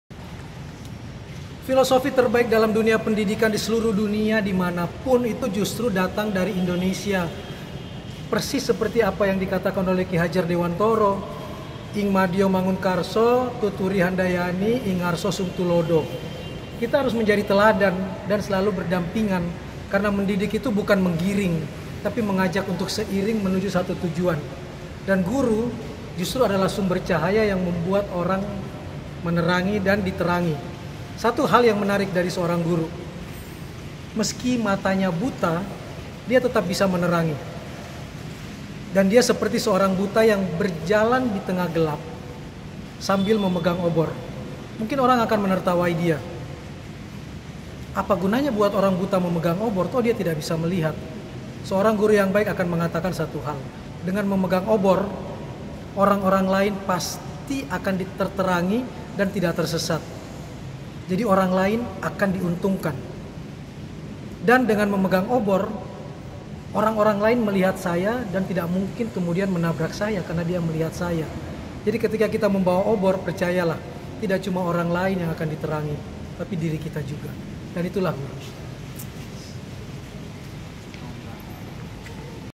Kang Maman: Beatiful Words
Kang Maman Shared at Paramount School Palembang (09/3/2018). His words and story motivated us as a educator, More over as  teachers. Thank you very much Kang.